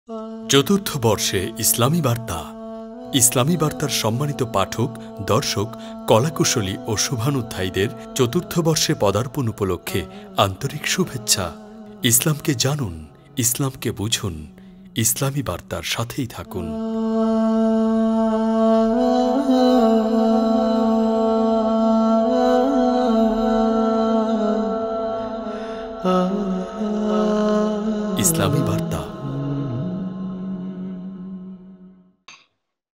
চতুর্থ বর্ষে ইসলামী বার্তার পদার্পণে সবাইকে শুভেচ্ছা
ইসলামী বার্তার চতুর্থ বর্ষপূর্তী উপলক্ষে সম্মানীত পাঠক, দর্শক, কলাকৌশলী ও শুভানুধ্যায়ীদের প্রতি আন্তরিক শুভেচ্ছা।